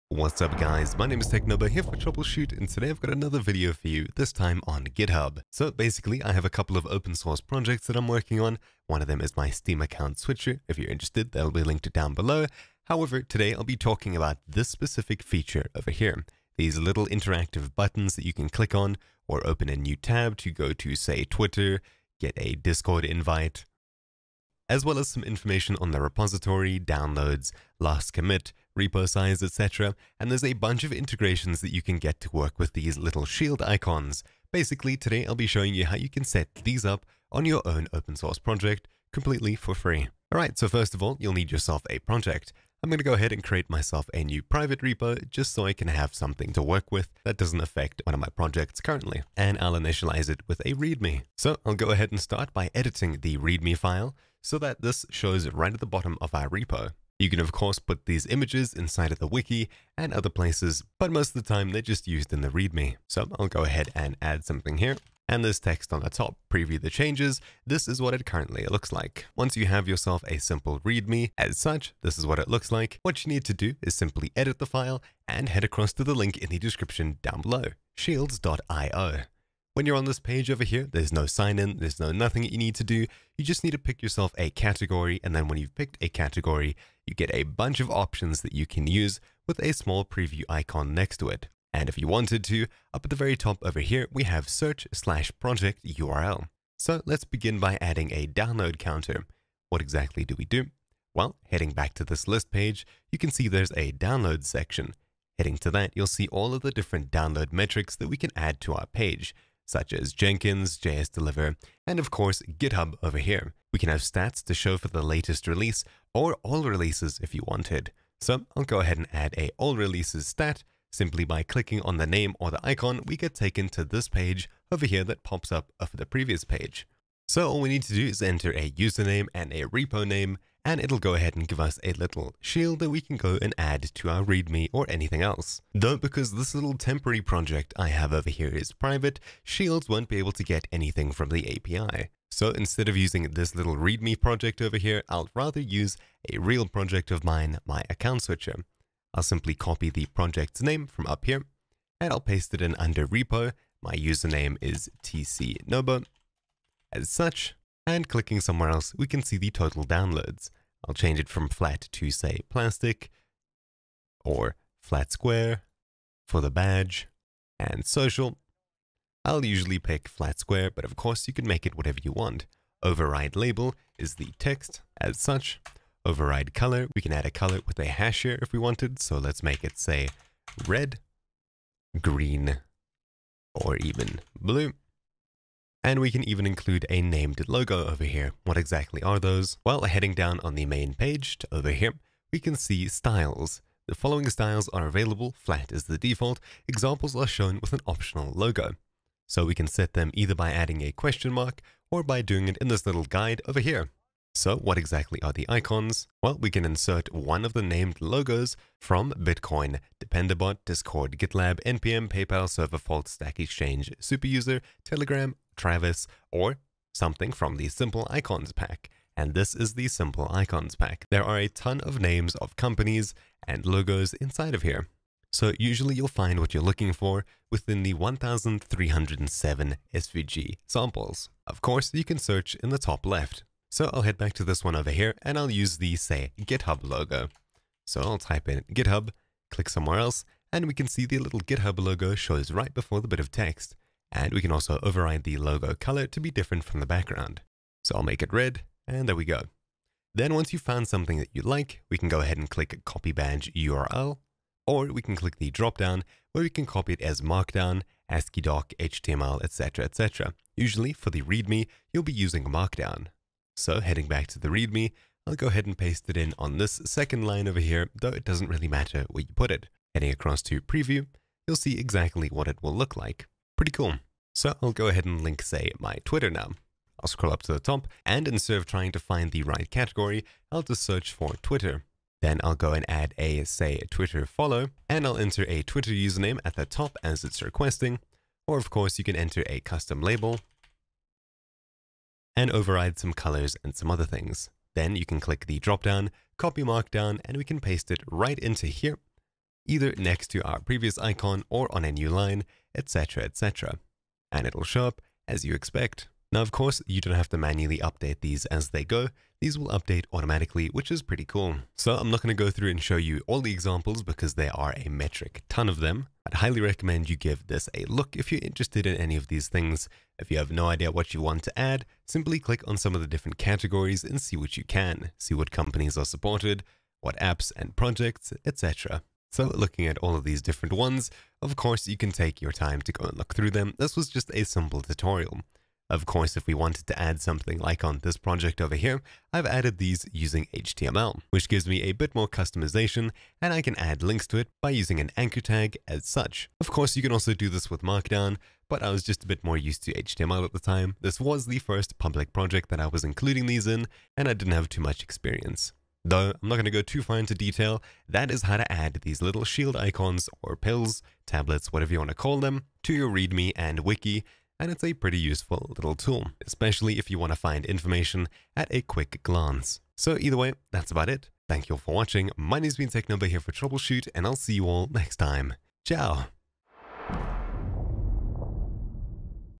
GitHub: How to add Shields | Easy, visible info on your projects
Seen those little icons talking about downloads, repo size and more? Well, this is how you add them to your project. This video covers the basics of creating them and adding them to your readme or wiki on GitHub, though they might work elsewhere as well.

Timestamps:
0:00 - Explanation
0:49 - Where you can place them
1:13 - Creating new shield icons
1:36 - Download counter icon
2:55 - Custom color
3:07 - Adding a logo
4:09 - Adding shields to Readme/Wiki
4:34 - Adding a Twitter Follow shieldv